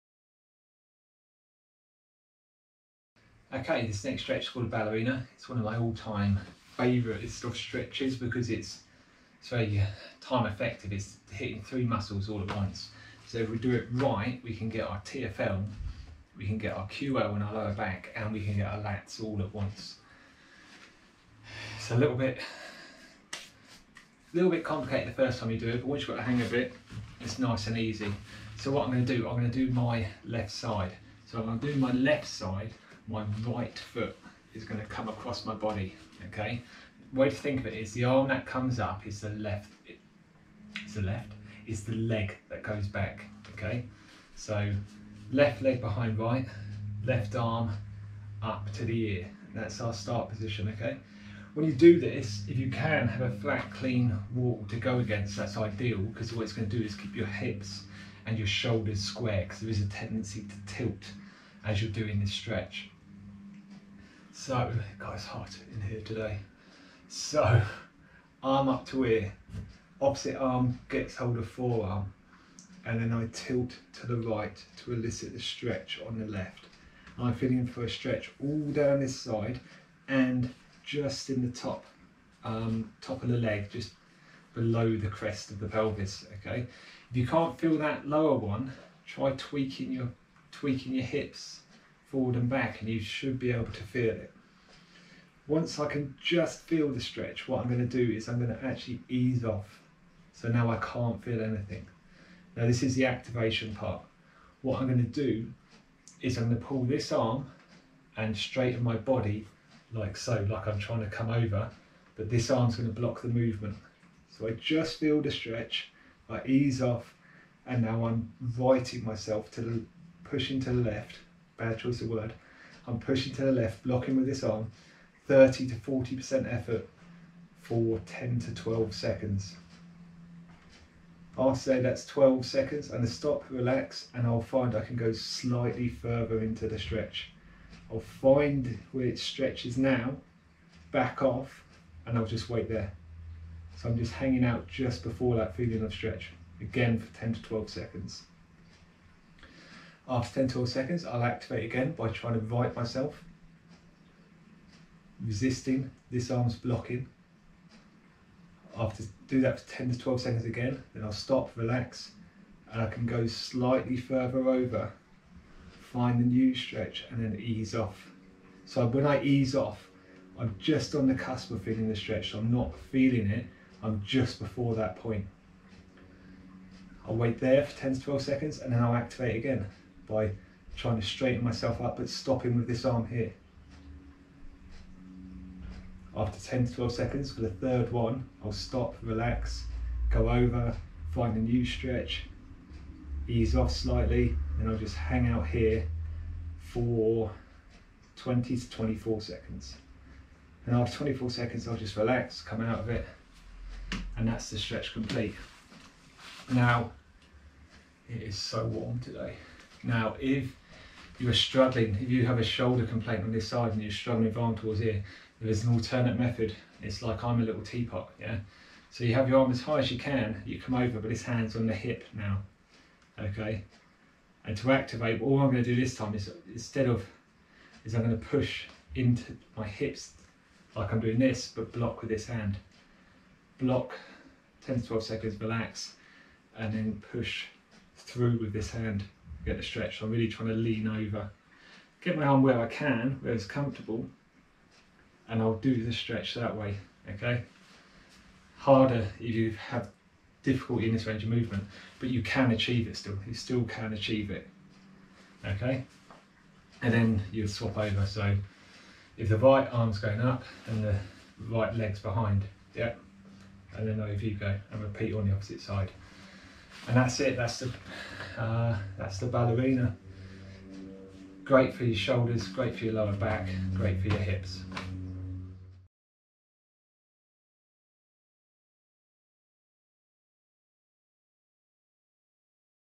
Ballerina Stretch
A great stretch that targets your TFL, QL and Lats all in one go.

This stretching technique uses a self administered MET (muscle energy technique) style.

MET utilises something called the GTO that regulates muscle tone. By stretching in this way you avoid activating the muscle spindles that can cause a protective contraction of the muscle.